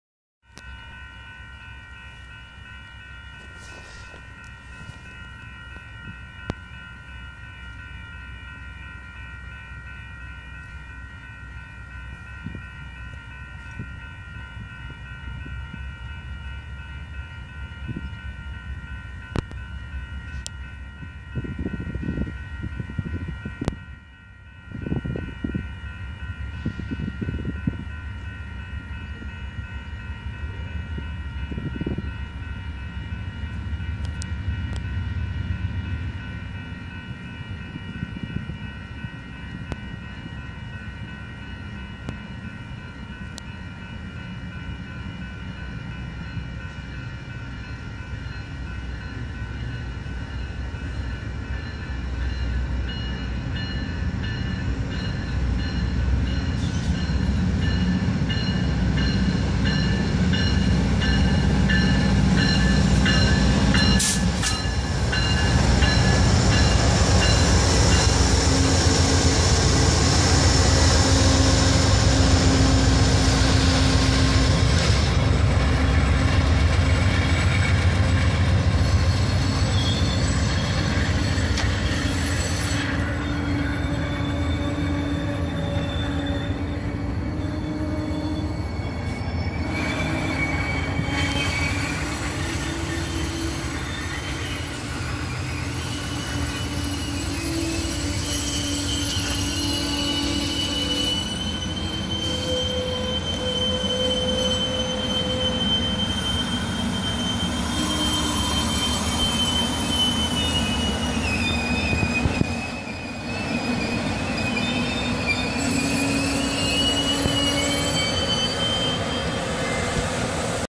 Amtrak 5 arriving into colfax
Amtrak 5 leads the zephyr into colfax